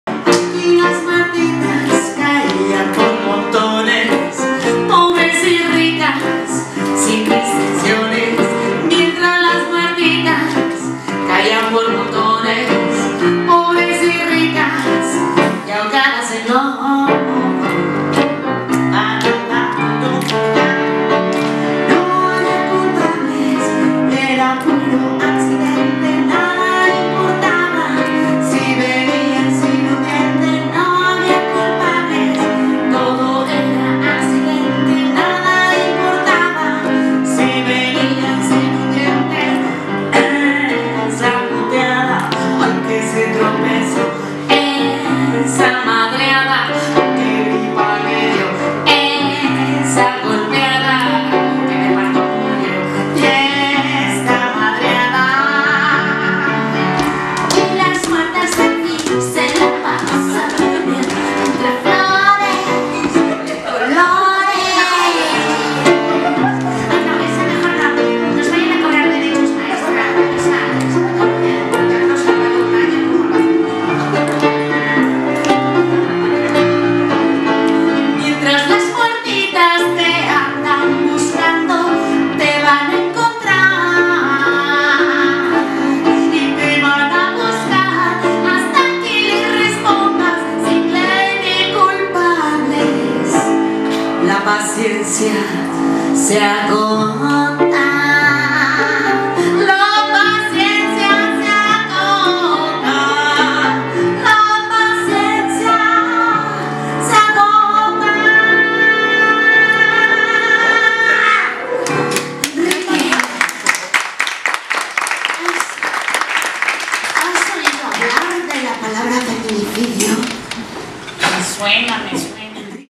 Nora Huerta en "Sé Lo que Hicieron Los Sexenios Pasados
Las Reinas Chulas presentan Sé Lo Que Hicieron Los Sexenios Pasados, espectáculo de Teatro Cabaret del género thriller pepsicológico, con Ceci Sotres, Nora Huerta y Alberto Eliseo.

Créditos
Autoría y dirección: Las Reinas Chulas
Con: Cecilia Sotres, Nora Huerta y Alberto Eliseo
Música en vivo: Analí Sánchez Neri